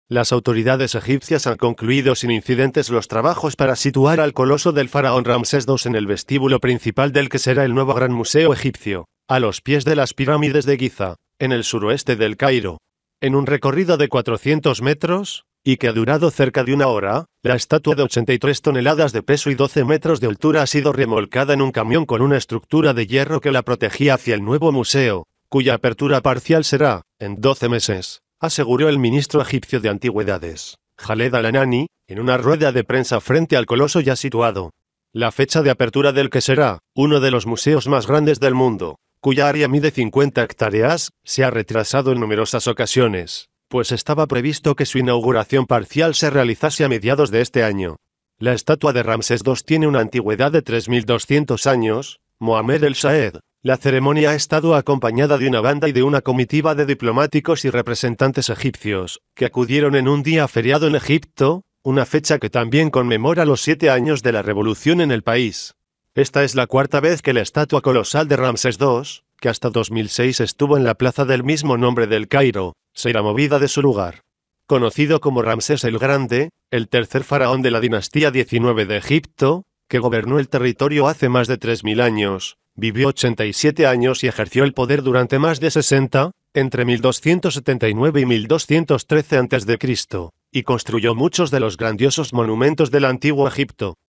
Trasladan estatua de Ramsés II de Giza al Gran Museo Egipcio Video
Las autoridades egipcias han concluido sin incidentes los trabajos para situar al coloso del faraón Ramsés II en el vestíbulo principal del que será el nuevo Gran Museo Egipcio, a los pies de las pirámides de Guiza, en el suroeste de El Cairo.

En un recorrido de 400 metros, y que ha durado cerca de una hora, la estatua de 83 toneladas de peso y 12 metros de altura ha sido remolcada en un camión con una estructura de hierro que la protegía hacia el nuevo museo, cuya apertura parcial será "en doce meses", aseguró el ministro egipcio de Antigüedades, Jaled al Anani, en una rueda de prensa frente al coloso ya situado.

La fecha de apertura del que será "uno de los museos más grandes del mundo", cuya área mide 50 hectáreas, se ha retrasado en numerosas ocasiones, pues estaba previsto que su inauguración parcial se realizase a mediados de este año.

La estatua de Ramsés II tiene una antigüedad de 3.200 años (Mohamed El-shahed)

La ceremonia ha estado acompañada de una banda y de una comitiva de diplomáticos y representantes egipcios, que acudieron en un día feriado en Egipto, una fecha que también conmemora los siete años de la revolución en el país.

Esta es la cuarta vez que la estatua colosal de Ramsés II, que hasta 2006 estuvo en la plaza del mismo nombre de El Cairo, será movida de su lugar.

Conocido como Ramsés el Grande, el tercer faraón de la Dinastía XIX de Egipto, que gobernó el territorio hace más de 3.000 años, vivió 87 años y ejerció el poder durante más de 60 (entre 1.279 y 1.213 a.C.) y construyó muchos de los grandiosos monumentos del Antiguo Egipto.